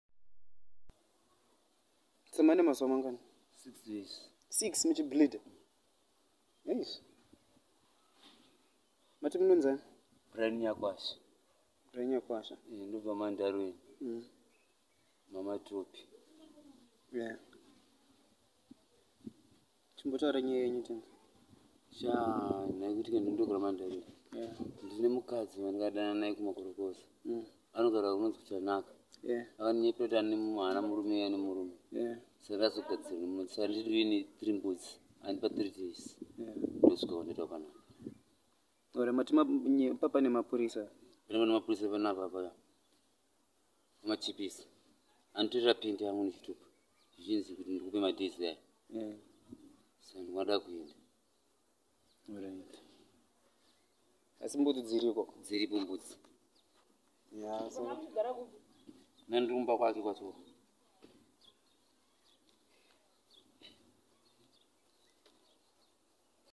MAN 'MENSTRUATES' AFTER AFFAIR
A Mt Darwin man is living in pain after spending six days bleeding through his genitals after bedding a married woman.
Brian Nyakwasva, who is a gold panner in Mazowe and stays in Matopi area, claims he did not know the woman was married.